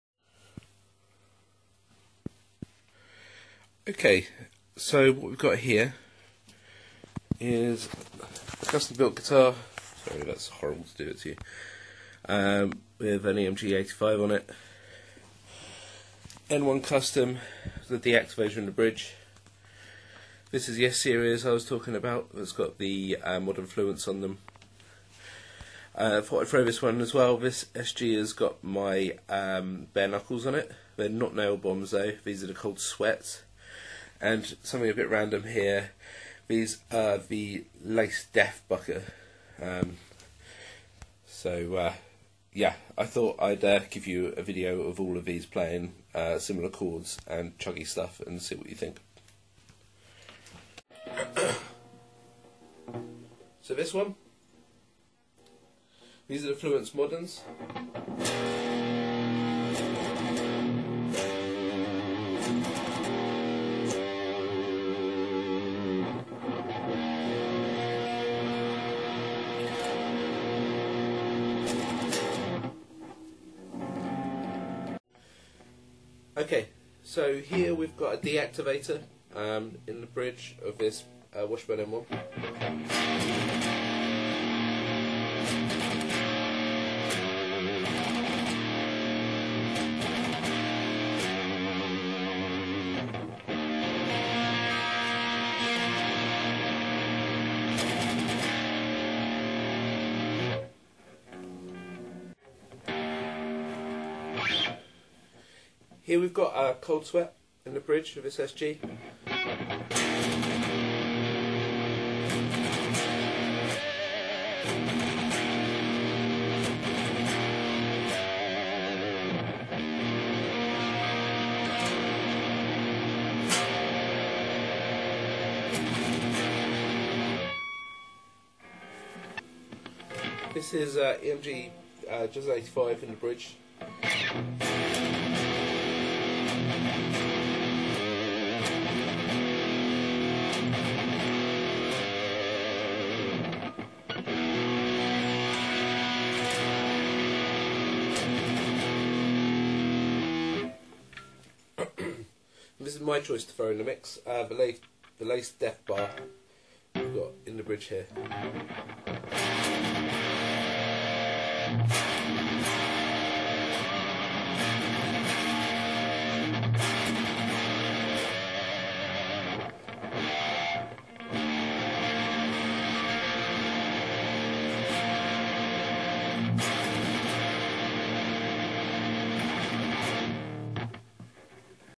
Pickup Comparison: Fishman, BKP, EMG, Dimarzio, Lace
Fishman Fluence Modern. DiMarzio D-Activator, Bare Knuckle Cold Sweat, EMG 85, Lace Death Bucker.